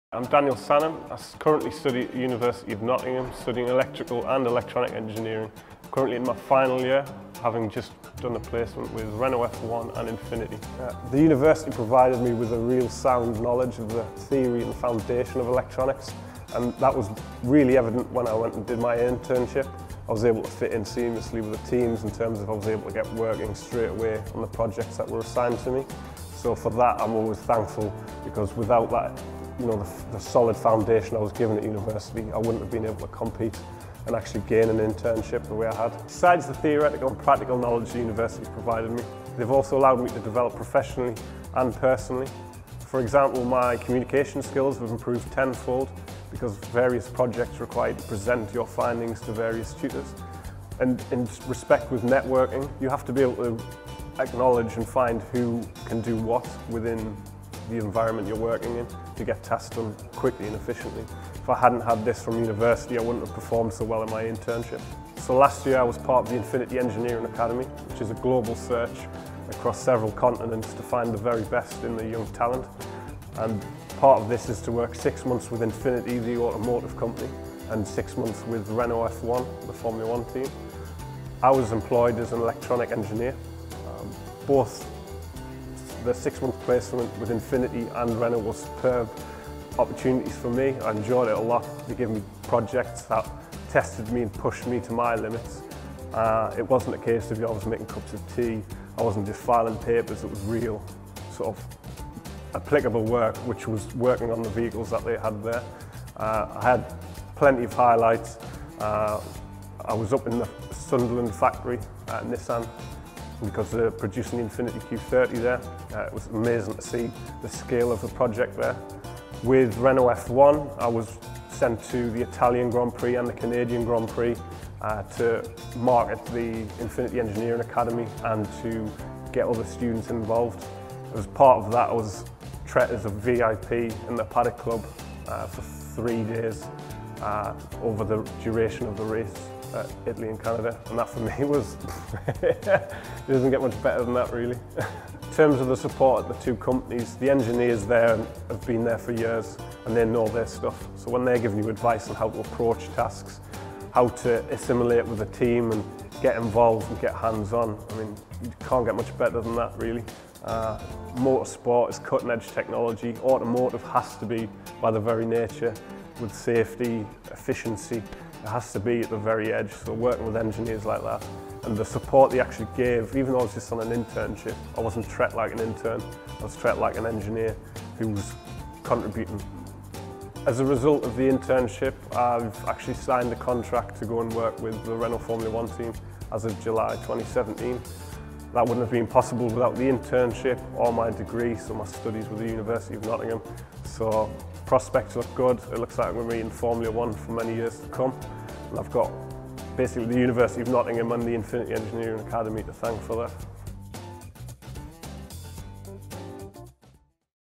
Daniel, Future Engineer at The University of Nottingham
Daniel Sanham, final undergraduate studying Electrical and Electronic Engineering student at The University of Nottingham, discusses his industrial placement at Renault F1 and the experience he has gained from the University having such strong links with industry.